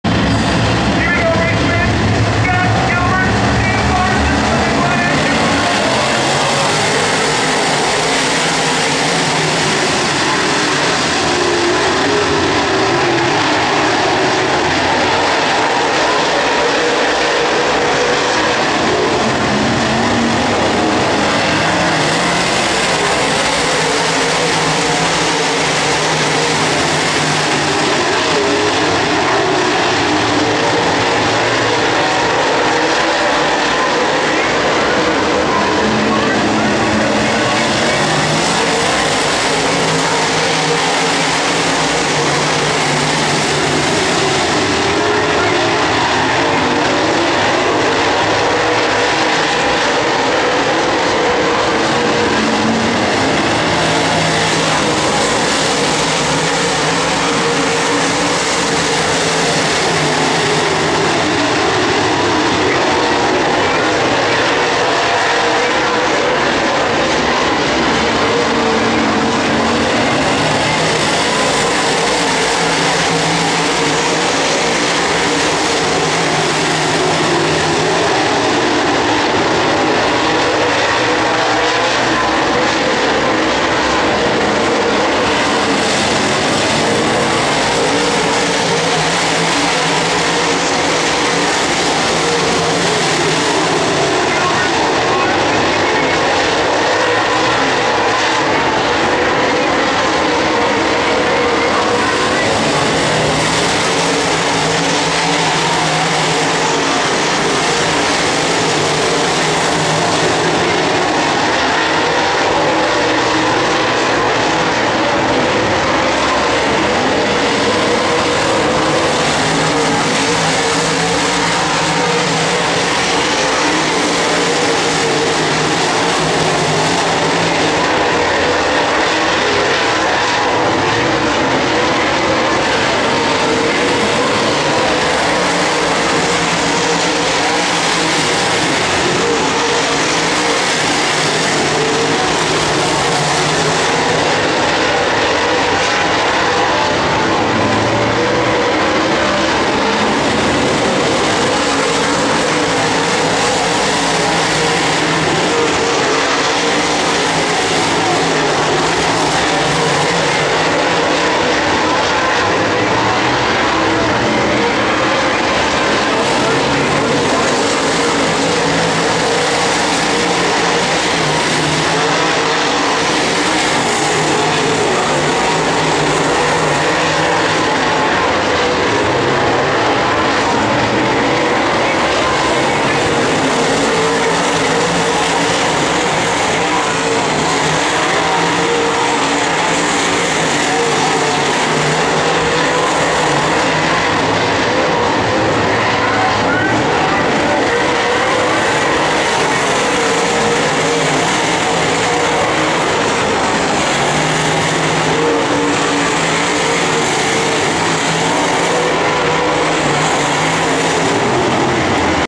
Legendary 100 Late Model Shootout
Friday night Late Model action at Cedar Lake Speedway.